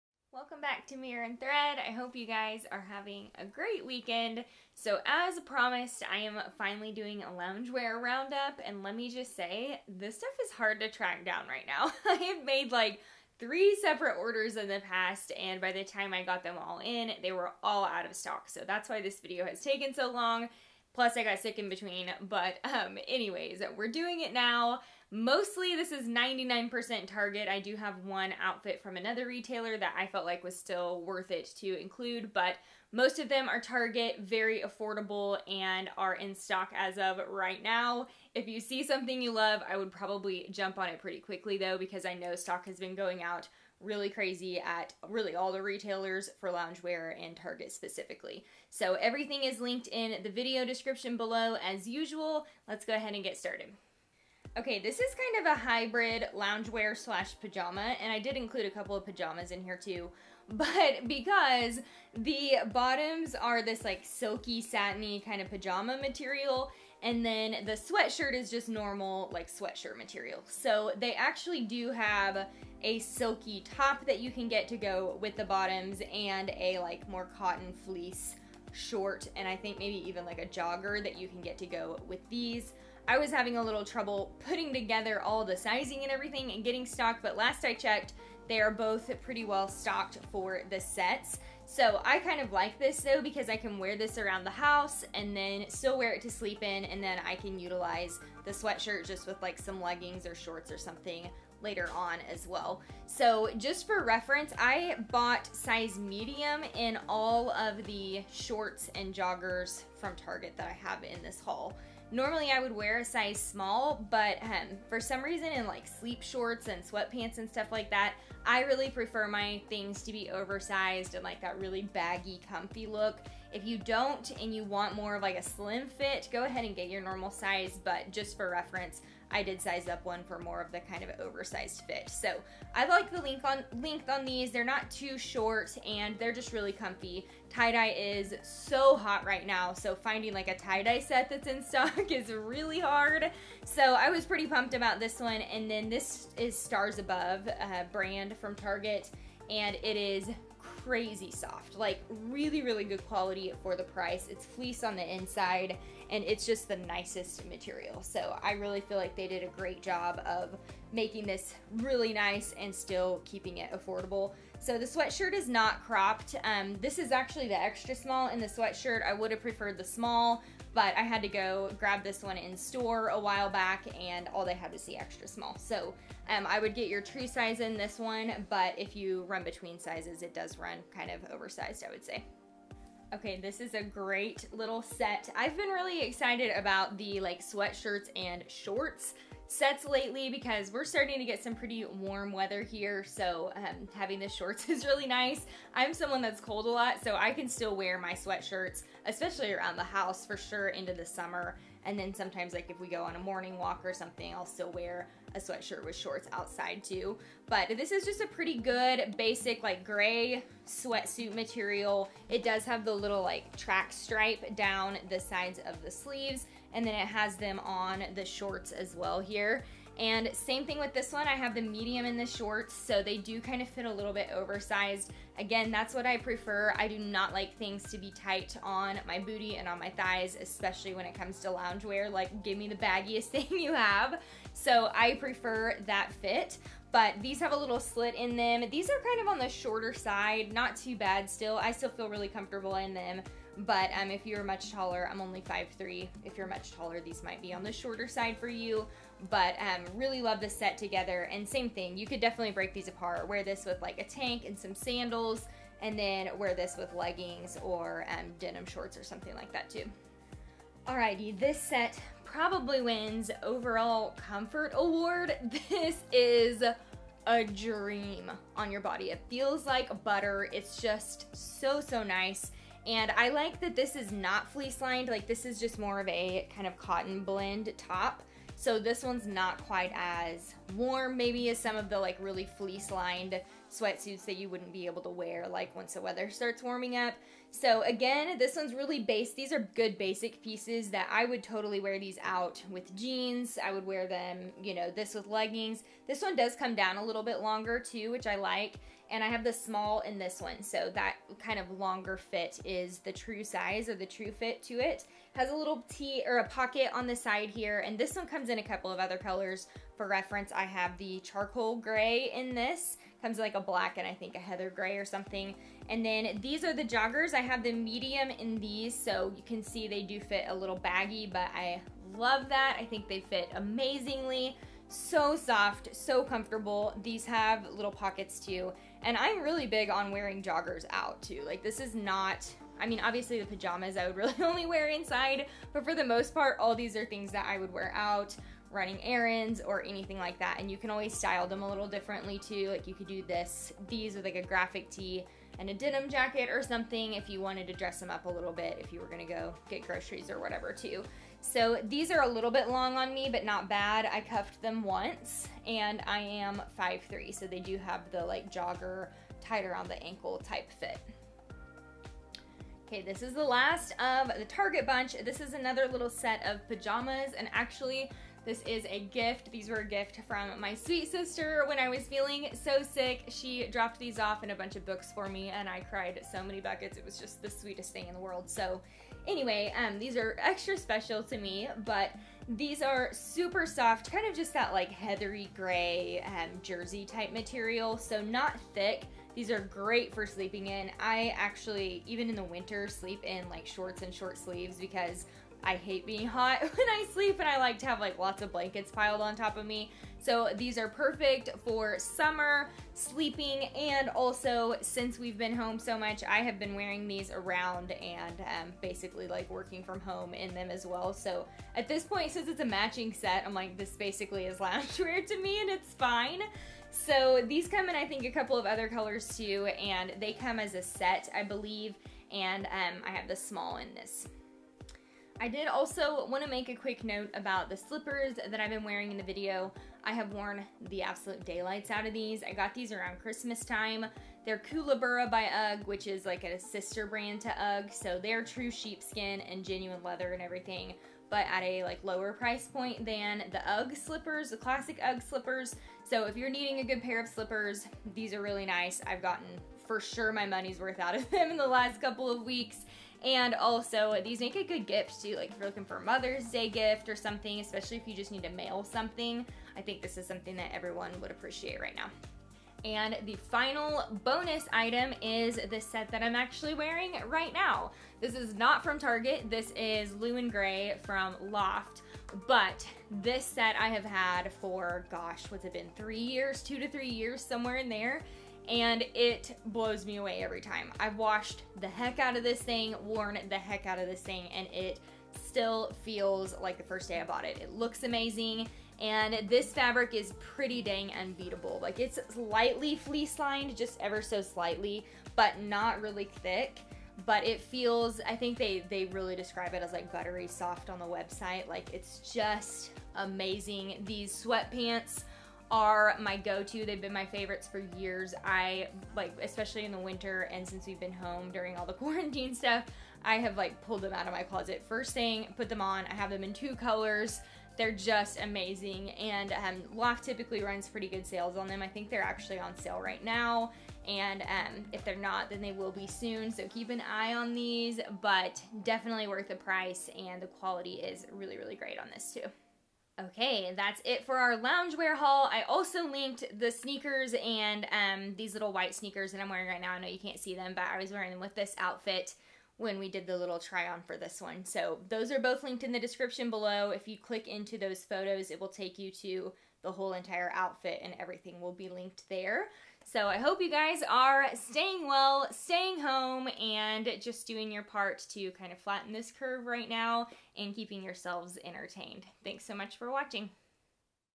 Target Loungewear Haul (Under $30!)
Shop ALL my daily outfits and sale notifications by following me on Like To Know it

Follow us on Amazon Live ►

My Measurements:

Height: 5’3
Jean size: 2/4
Bra size: 34B
Shoe size: 6.5

Outfit 1:
Tie dye sweatshirt and silk pajama shorts- Click to shop the FULL outfit 👉🏻

Outfit 2:
Dark gray sweatshirt and joggers matching set, ugg slippers- Click to shop the FULL outfit 👉🏻

Outfit 3:
Gray sweatshirt and sweat shorts with white stripes, tan sneakers- Click to shop the FULL outfit 👉🏻

Outfit 4:
Gray pajama set, ugg slippers- Click to shop the FULL outfit 👉🏻

Outfit 5:
Black loungewear set, white sneakers, headband- Click to shop the FULL outfit 👉🏻